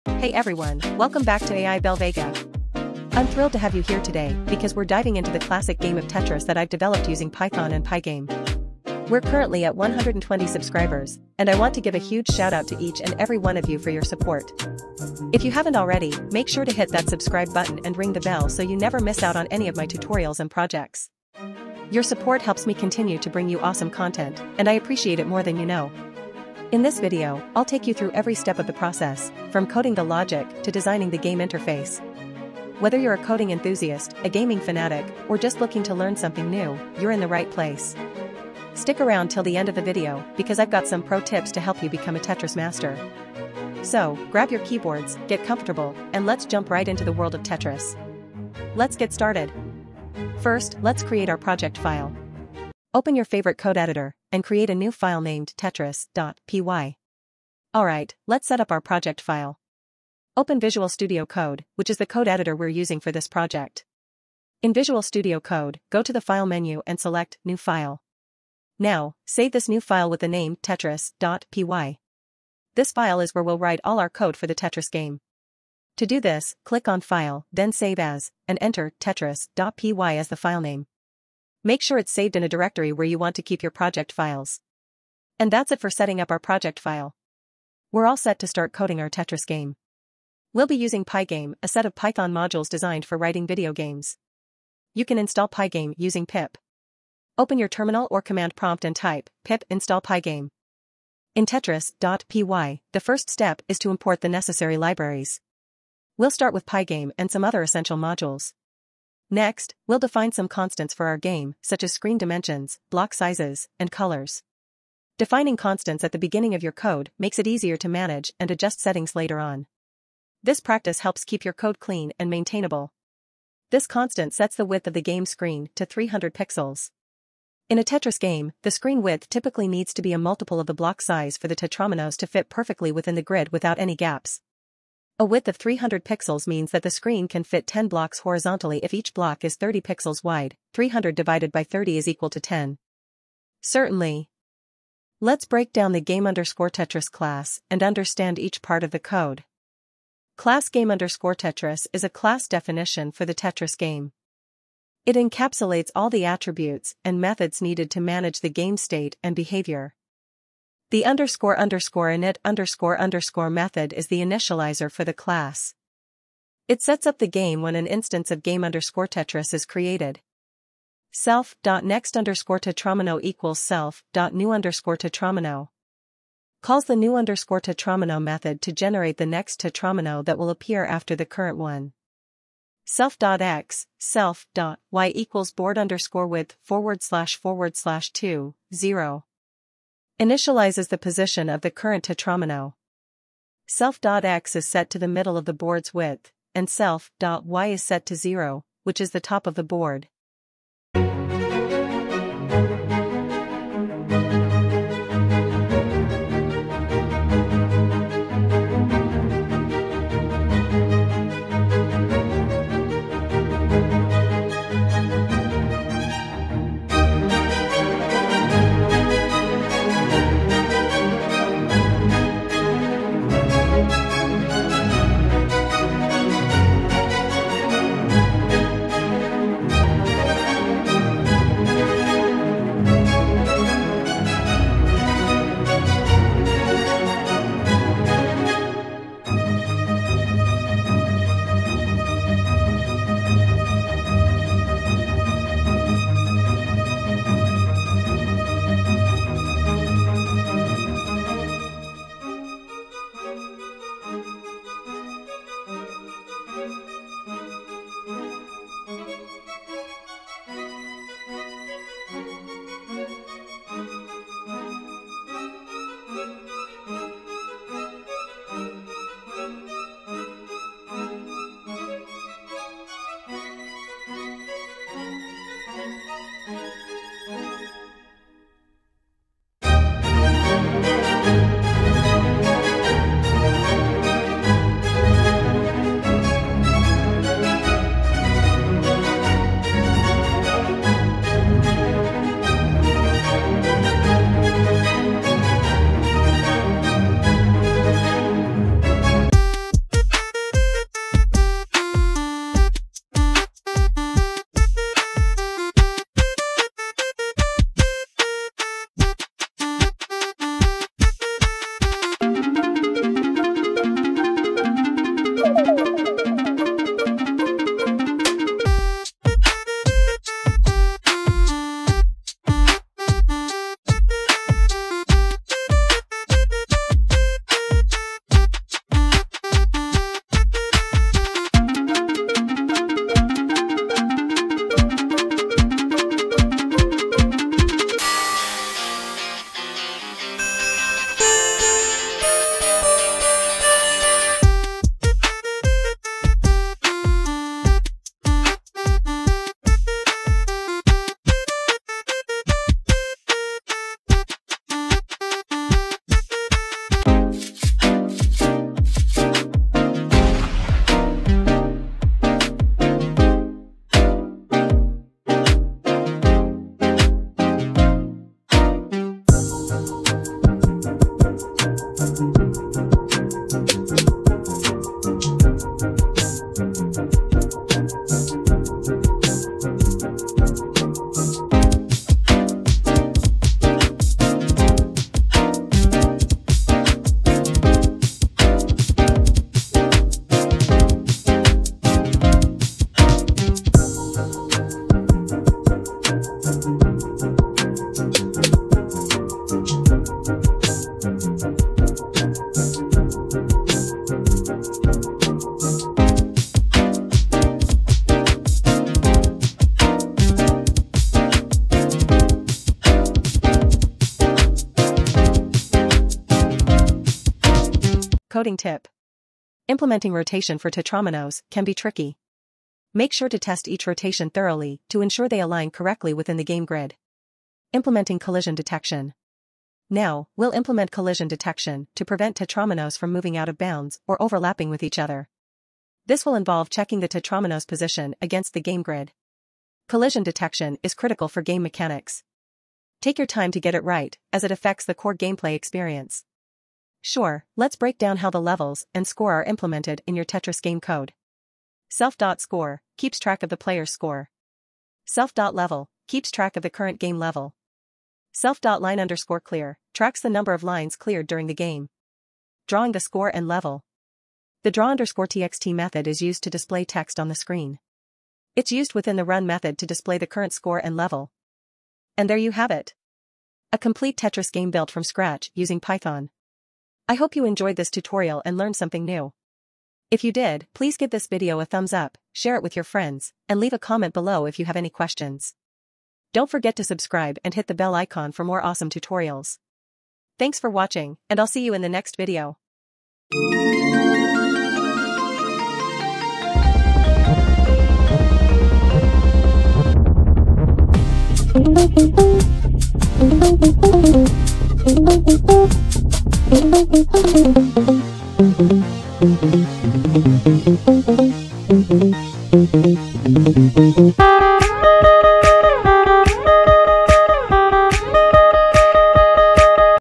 Build a Classic Tetris Game with Python! Full Tutorial for All Levels 🎮
Learn how to build a classic Tetris game with Python in this full tutorial for beginners and advanced. Follow along and create your own game from scratch! 🎮 Welcome to our full tutorial on building a classic Tetris game with Python! Whether you're a beginner or looking to sharpen your coding skills, this video will guide you through the entire process step-by-step.

In This Video, You Will Learn:
✅Installing and using Pygame
✅Creating the game window and grid
✅Designing and implementing Tetris shapes
✅Coding the game logic and controls
✅Adding scoring and levels

✅Beginner-Friendly: Clear explanations and no prior experience needed.
✅Hands-On Learning: Code along with me to build your own Tetris game.
✅Comprehensive: Covers everything from setup to a fully playable game.
✅Tips and Tricks: Helpful coding tips to make your development process smoother.

Coding Tips Included:
✅How to structure your game code for readability and maintainability.
✅Techniques for handling user input and game state management.
✅Best practices for using Pygame efficiently.

Chapters:
0:00 Introduction
0:58 Setting up the environment
1:06 Creating the game window
1:41 Designing Tetris shapes
5:42 Implementing game controls
6:21 Adding scoring and level
7:22 Final touches and tips